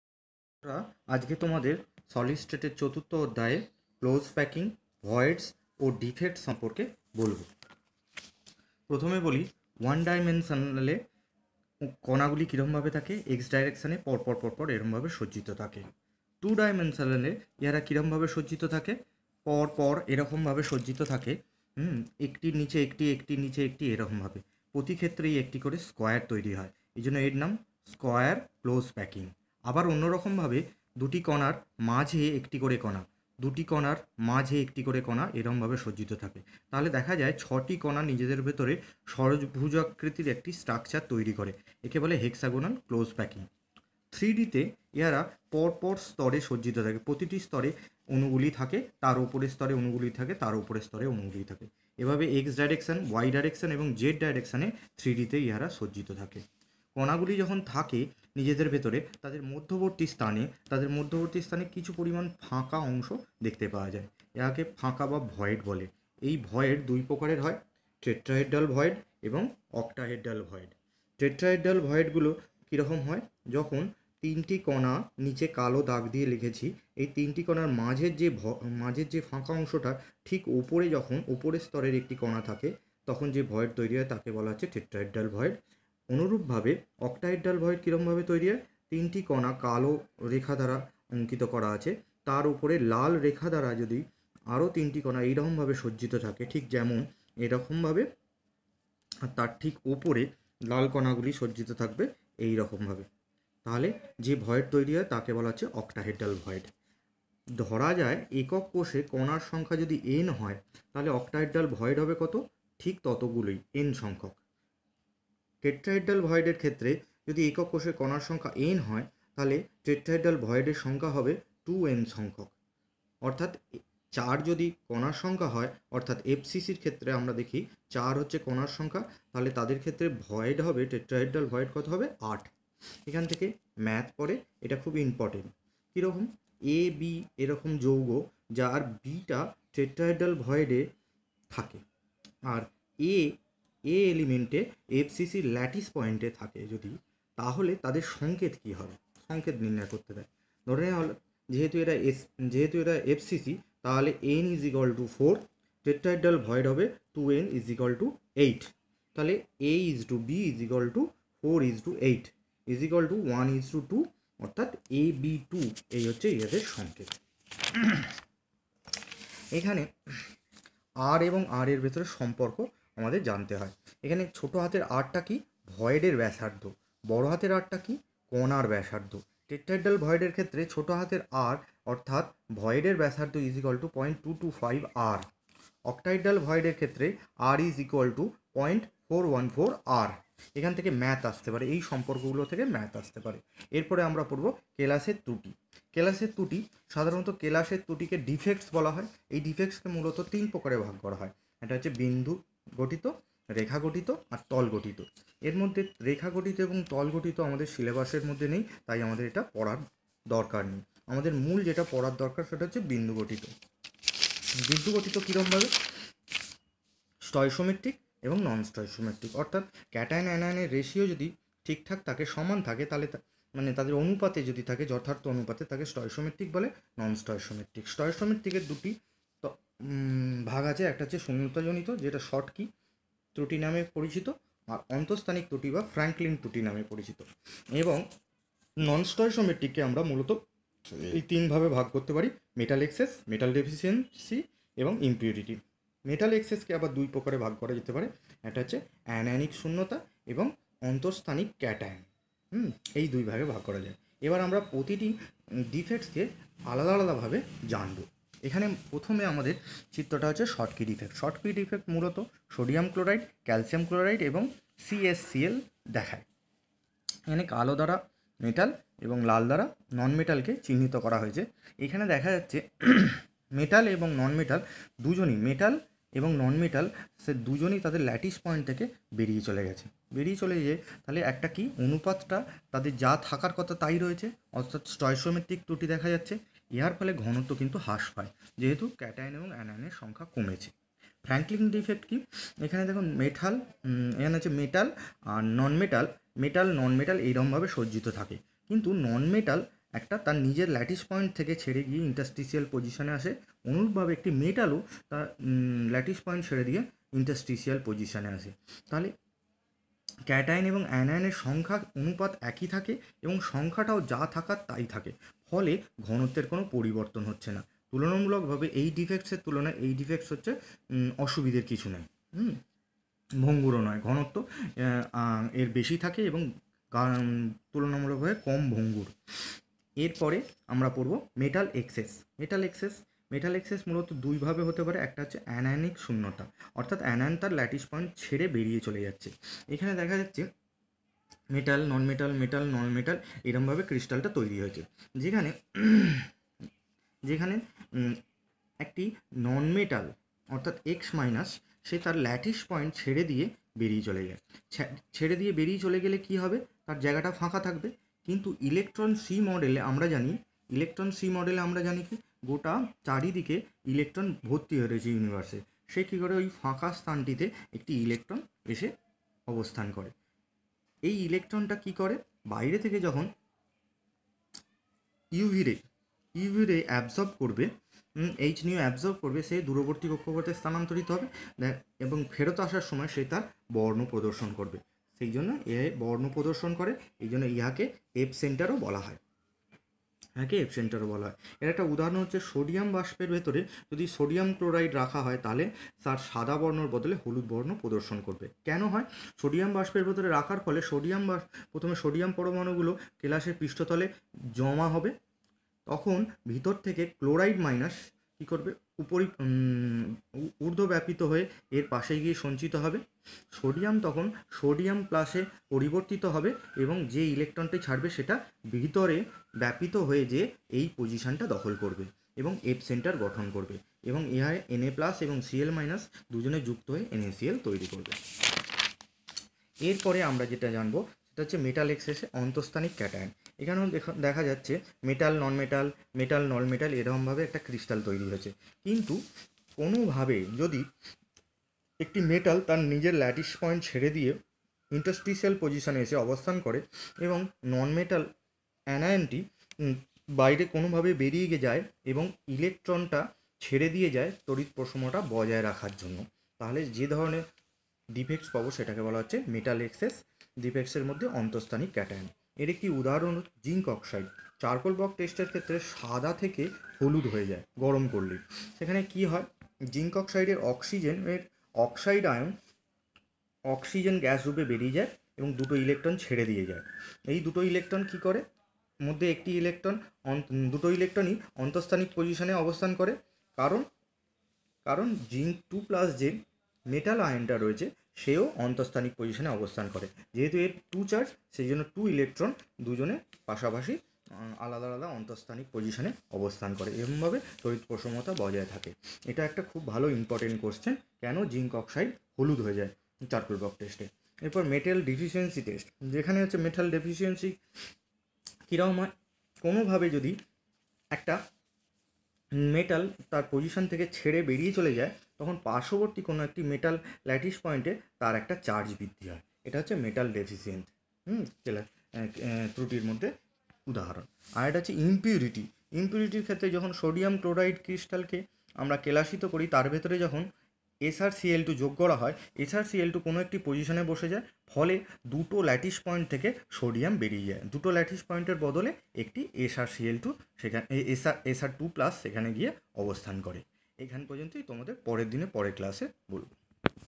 Class Xii, Chemistry, Solid state/voids/defects
Class Xii, Chemistry, Solid state/voids/defects by Atanu Malick MSc in Applied Chemistry, B.Ed.